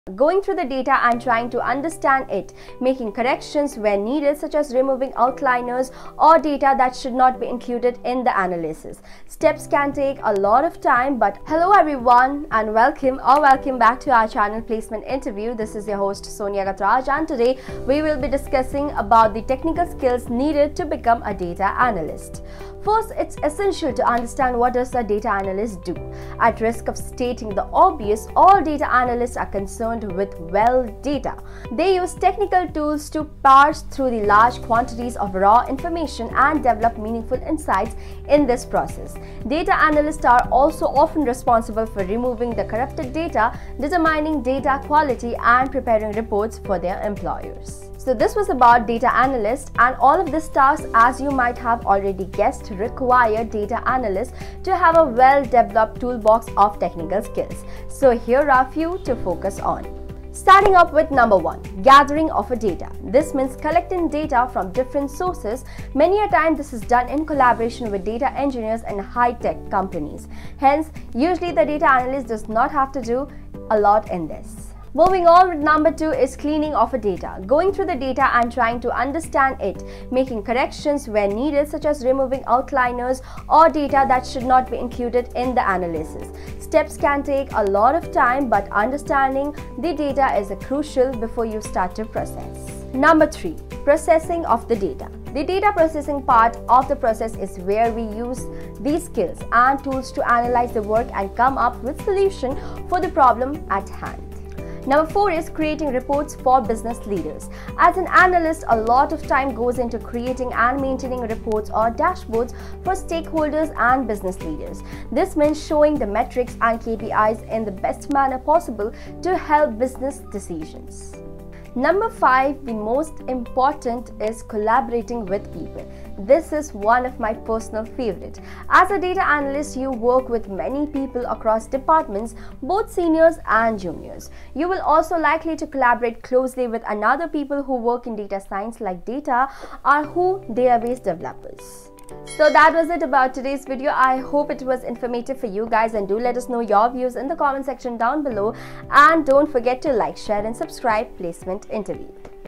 Data Analyst Roles and Responsibilities | Interview Questions
First, it's essential to understand what a data analyst does. At risk of stating the obvious - all data analysts are concerned with, well, data. They use technical tools to parse through large quantities of raw information and develop meaningful insights in the process. Data analysts are also often responsible for removing corrupted data, determining data quality, and preparing reports for their employer.

All of these tasks, as you might have + already guessed, require data analysts to have a well-developed toolbox of technical skills. Here are a few to focus on.
(1) Gathering Data

This means collecting data from different sources. Many a times this is done in collaboration with data engineers and architects hence usually the data analyst doesn't have to do a lot in this.